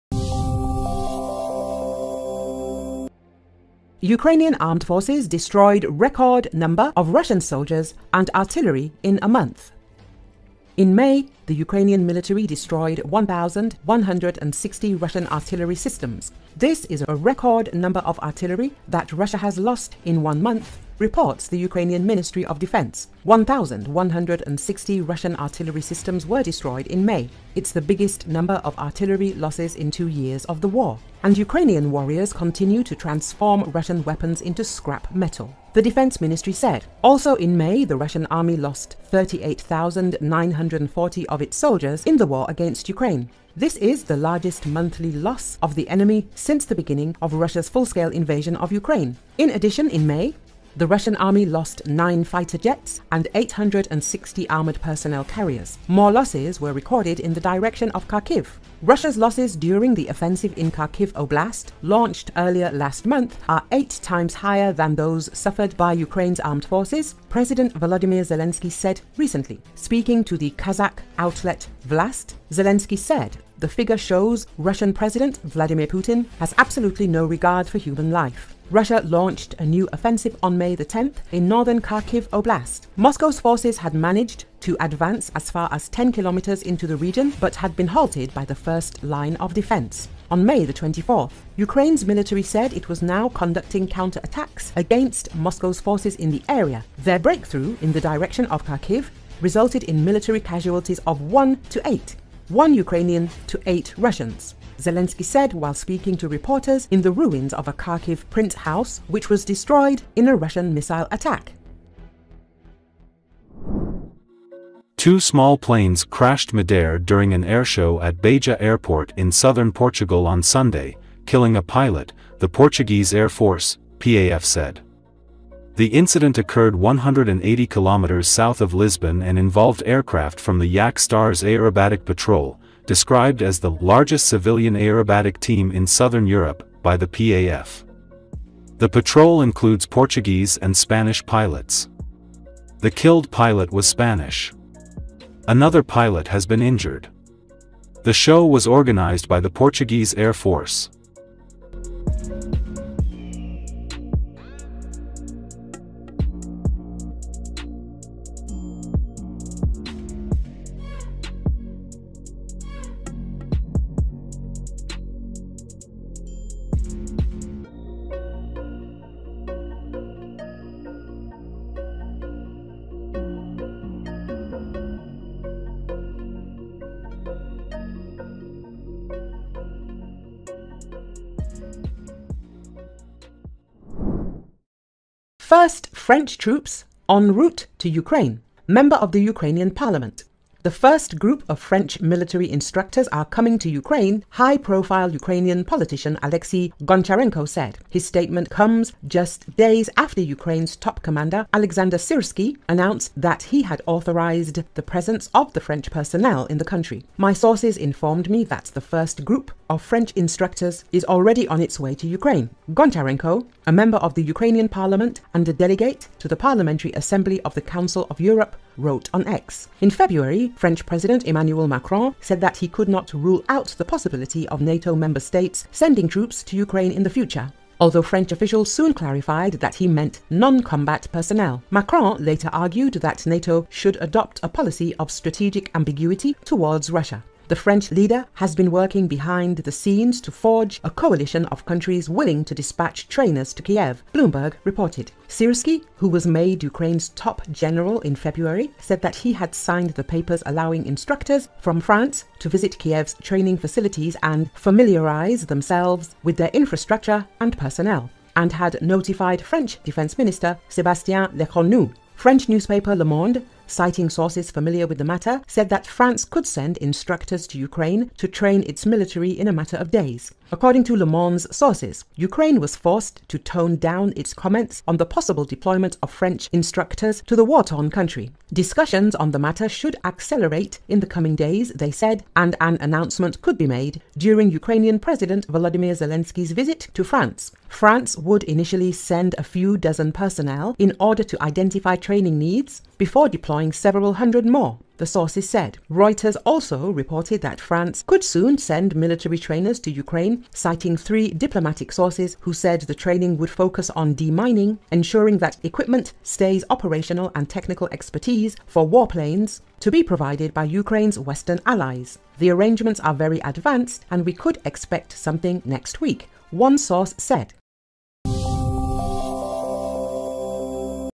Ukrainian Armed Forces destroyed record number of Russian soldiers and artillery in a month
In May, the Ukrainian military destroyed 1,160 Russian artillery systems. This is a record number of artillery that Russia has lost in one month, reports the Ukrainian Ministry of Defense.
“1160 Russian artillery systems were destroyed in May. It's the biggest number of artillery losses in two years of the war. And Ukrainian warriors continue to transform Russian weapons into scrap metal,” the Defense Ministry said.
Also in May, the Russian army lost 38,940 of its soldiers in the war against Ukraine. This is the largest monthly loss of the enemy since the beginning of Russia's full-scale invasion of Ukraine.
In addition, in May, the Russian army lost 9 fighter jets and 860 armored personnel carriers. More losses were recorded in the direction of Kharkiv.
Russia's losses during the offensive in Kharkiv Oblast launched earlier last month are eight times higher than those suffered by Ukraine's armed forces, President Volodymyr Zelensky said recently.
Speaking to the Kazakh outlet Vlast, Zelensky said the figure shows Russian president  Vladimir Putin "has absolutely no regard for human life."
Russia launched a new offensive on May 10 in northern Kharkiv Oblast. Moscow's forces had managed to advance as far as 10 kilometers into the region but had been halted by the first line of defense. On May 24, Ukraine's military said it was now conducting counterattacks against Moscow's forces in the area.
"Their breakthrough in the direction of Kharkiv, resulted in military casualties of 1 to 8 – one Ukrainian to eight Russians," Zelensky said while speaking to reporters in the ruins of a Kharkiv print house which was destroyed in a Russian missile attack.